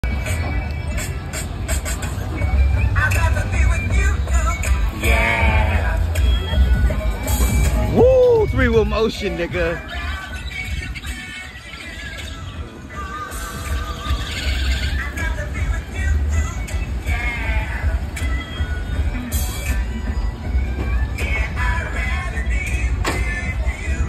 3 Wheel Motion LowRider 👀
The title says it all!! Dope LowRider downtown, Los Angeles…